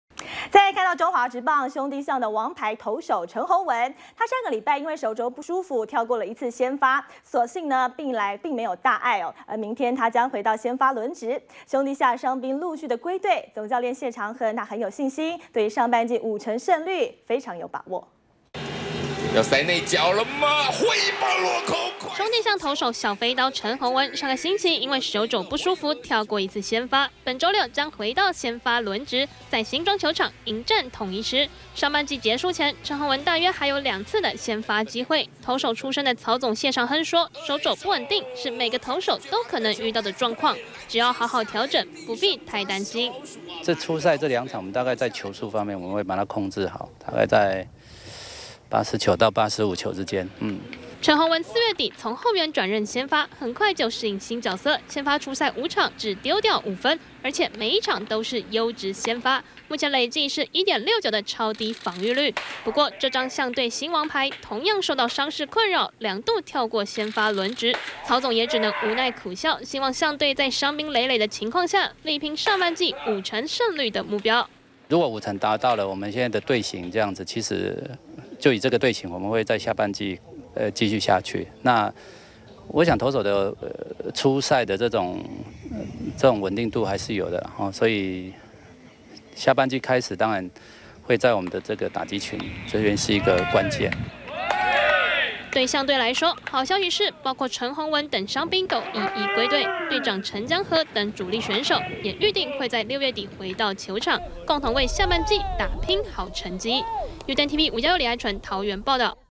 【2013.06.14】陳鴻文手肘無大礙 周六戰統一 -udn tv
中華職棒兄弟象的王牌投手陳鴻文，上周因為手肘不舒服，跳過一次先發，所幸並無大礙，周六將回到先發輪值。隨著傷兵陸續歸隊，總教練謝長亨希望象隊可以達到上半季五成勝率的目標，再以完整的陣容力拚下半季。